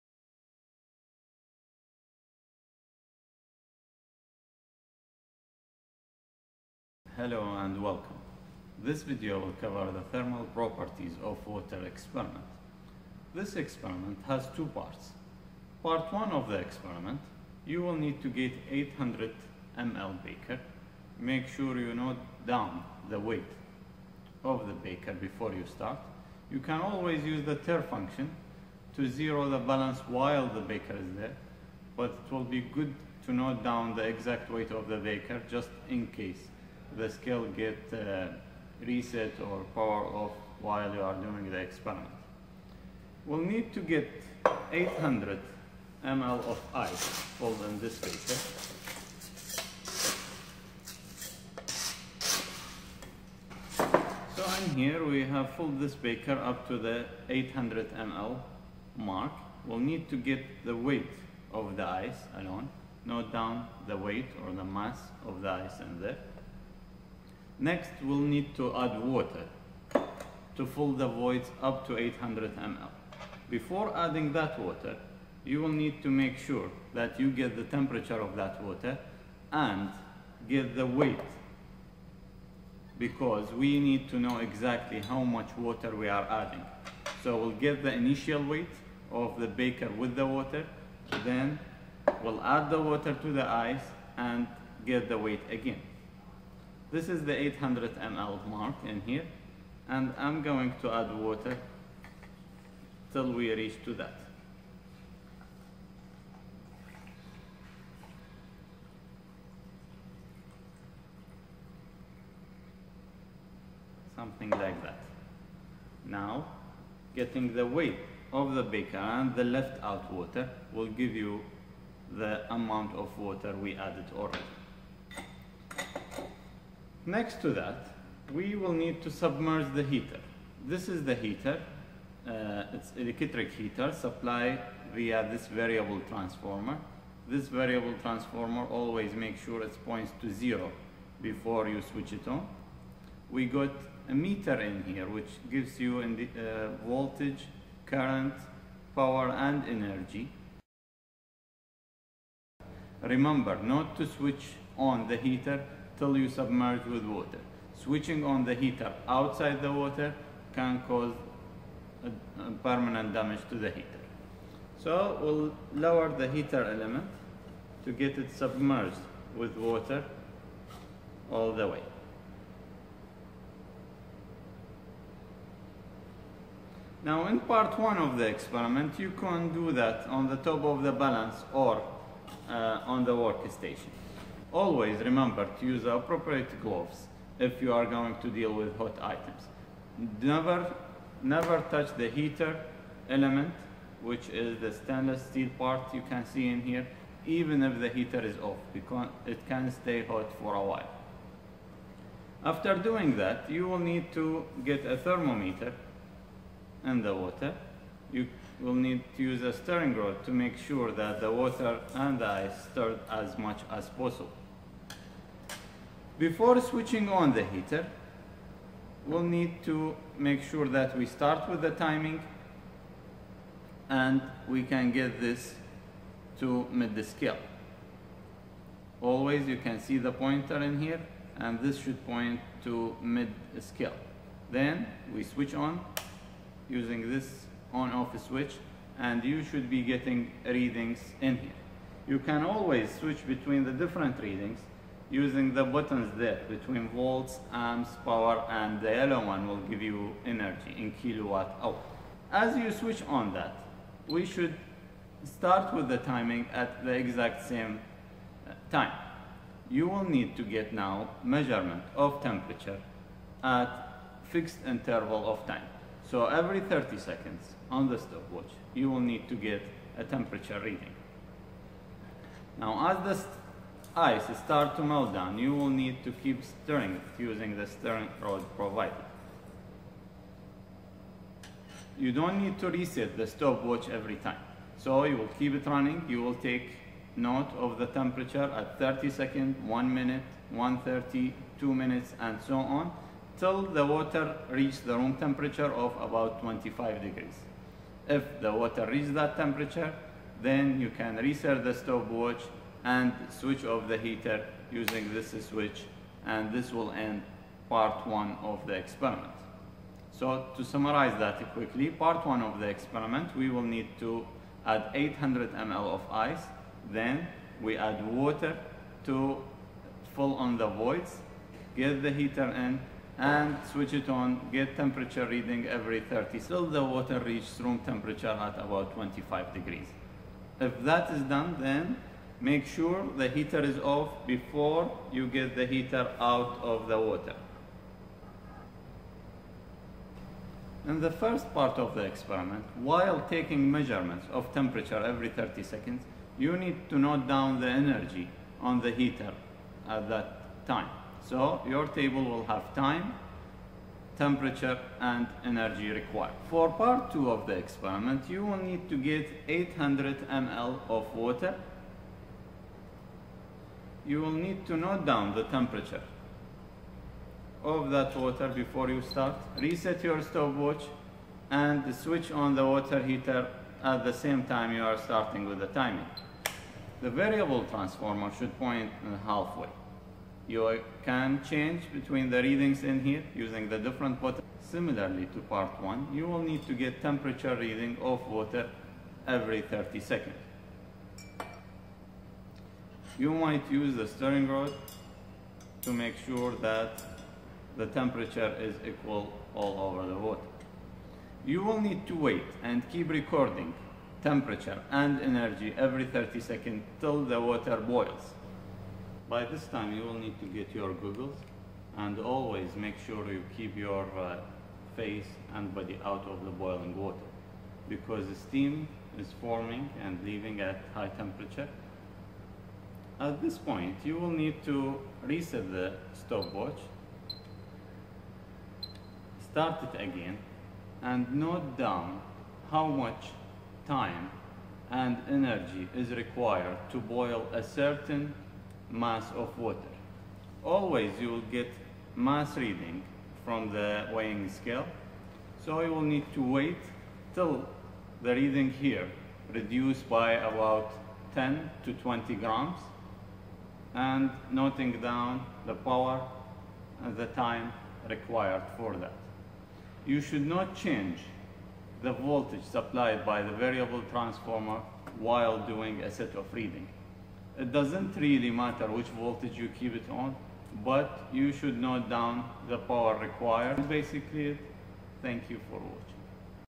Determination of the Thermal Properties of Water HWUDC Vlab
Measure the specific heat capacity, the latent heat of fusion and the latent heat of vaporisation of water at standard room conditions.
HWUDC Vlab